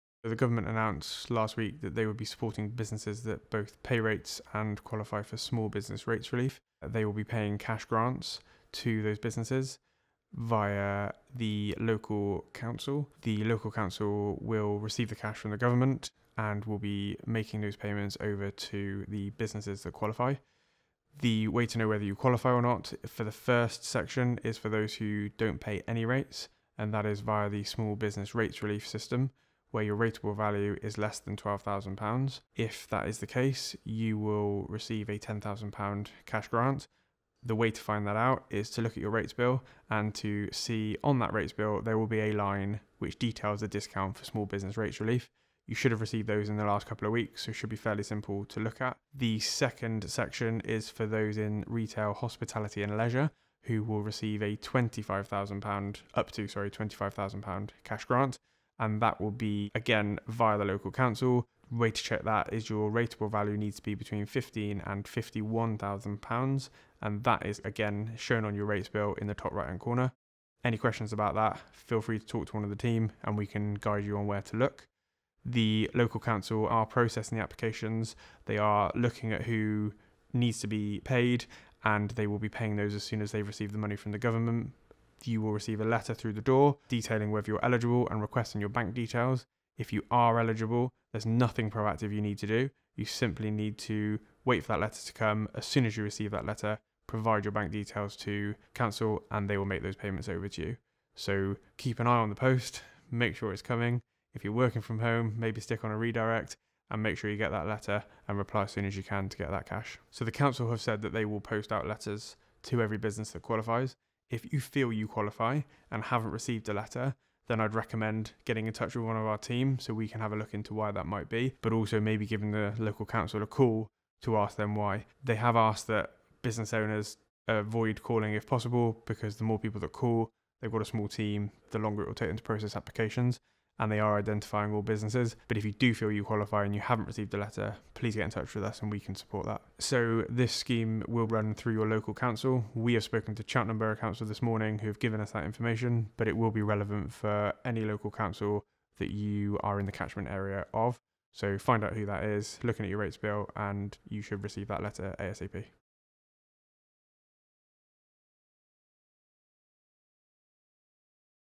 Nephos - Business Grants Update
Hear how the business rates grants will be calculated and how you can find out if you are eligible.